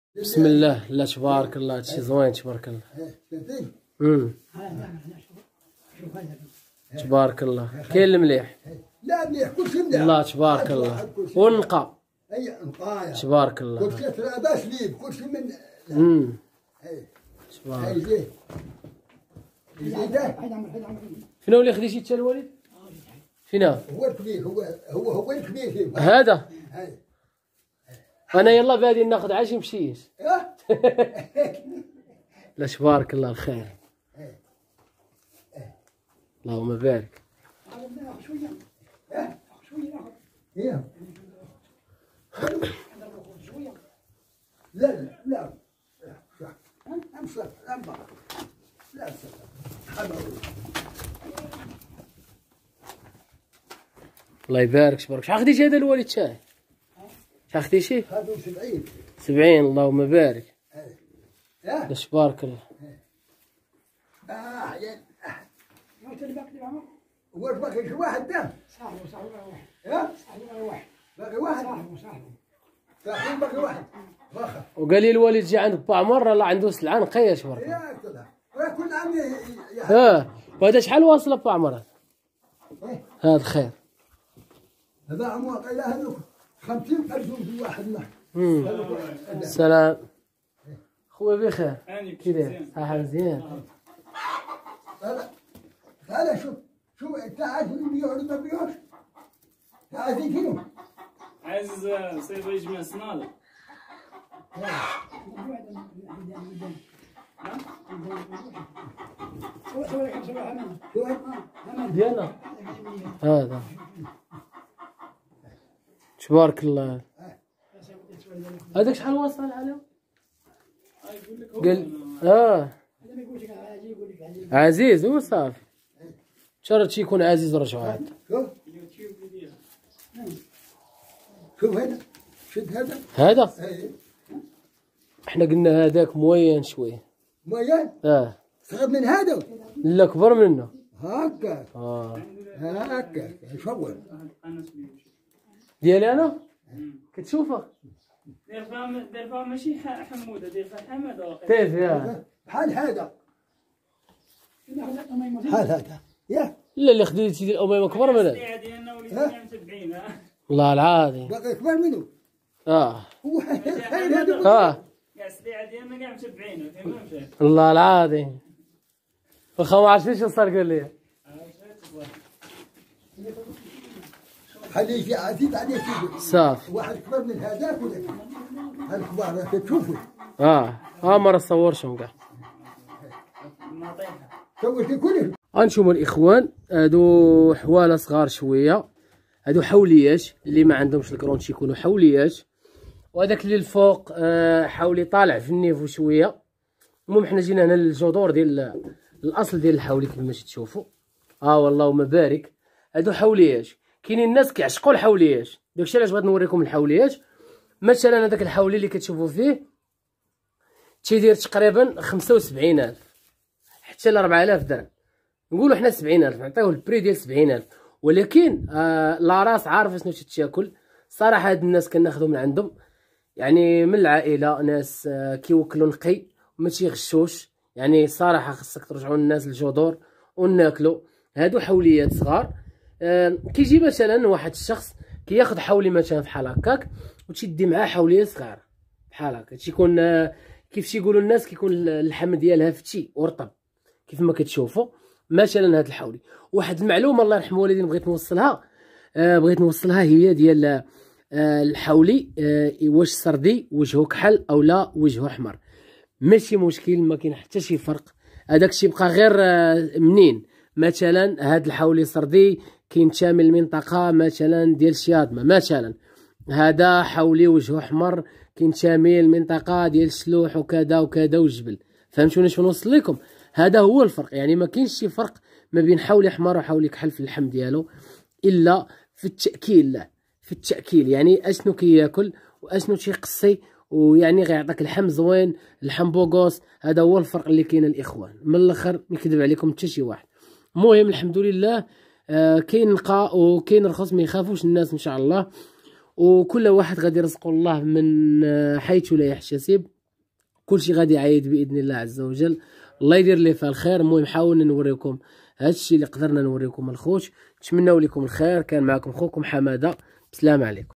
شوفو لحولي صردي ديال هذ لعام
الحمدلله  الكبش موجود  ديال العيد كبير  الله يرزكم كاملين يارب